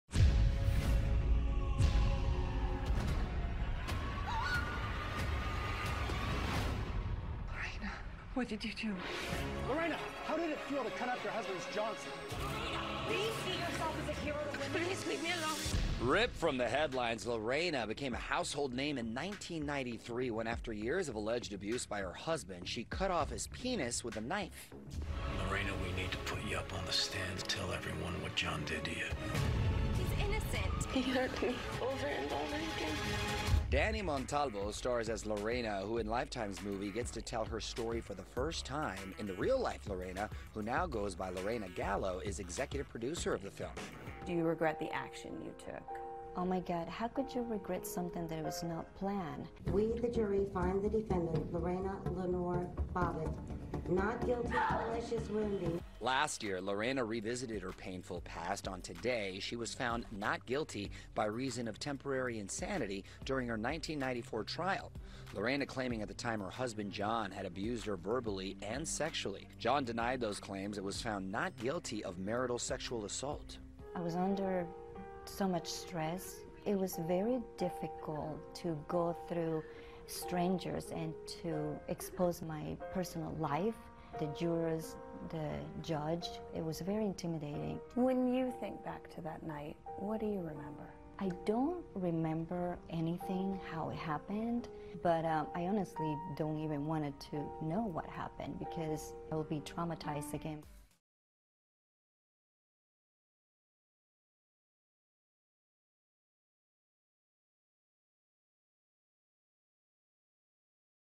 Lorena Bobbitt Tells Her Story In New Lifetime Movie
There was no bigger story in the '90s than of Lorena Bobbit, who took drastic measures against her husband John after years of abuse. Now, she tells her story in the new Lifetime movie, "I Was Lorena Bobbitt." Access Hollywood takes a look back at Lorena’s journey. "I Was Lorena Bobbitt" premieres May 25 at 8/7c on Lifetime Movie Network.